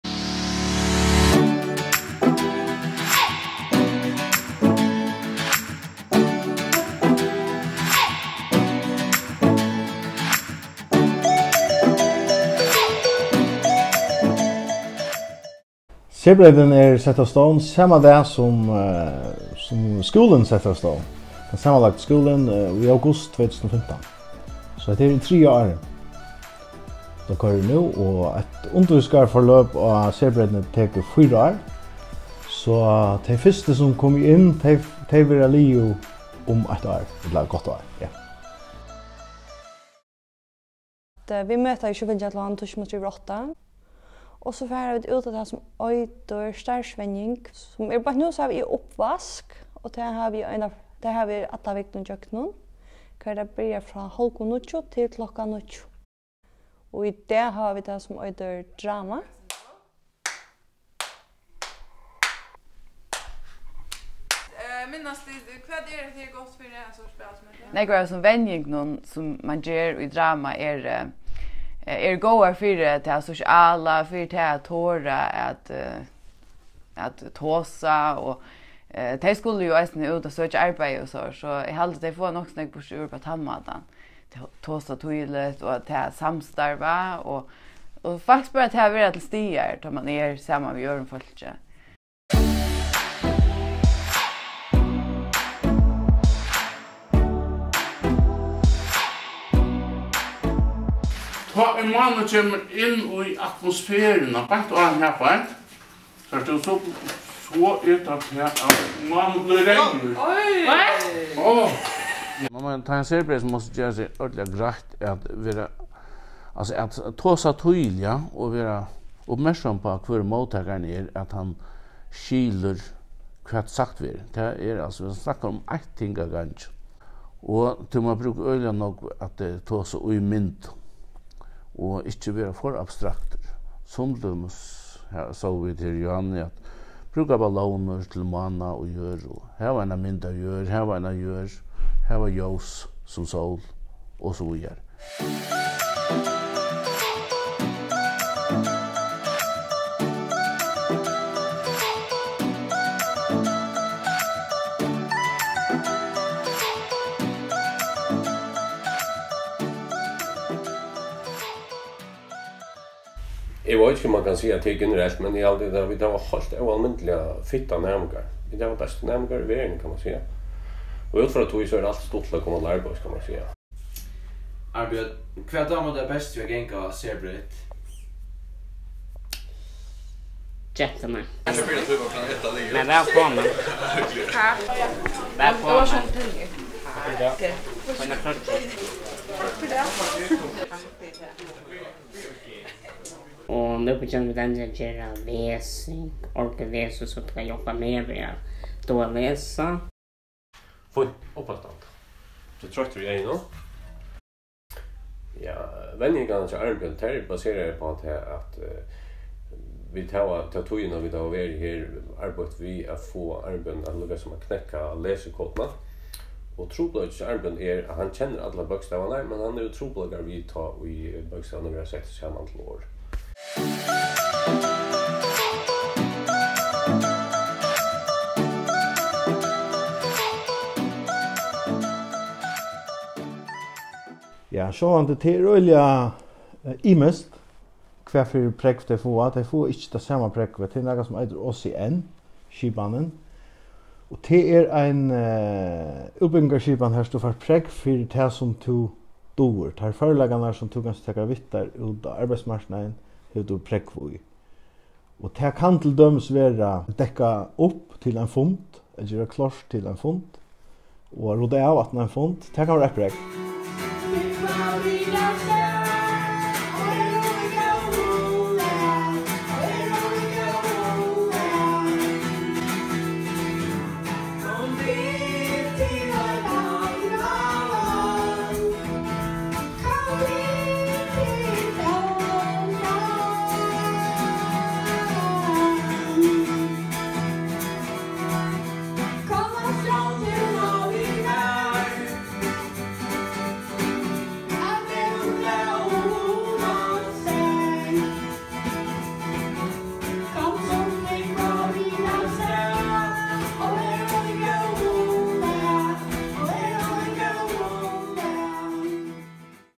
Serbreytin á Kambsdali
Hetta er ein stuttur dokumentarfilmur um serbreytina, sum Benjamin E. Tvørfoss, Dánial Danielsen, Bjarki Finnson og Bárður á Reynatrøð hava gjørt.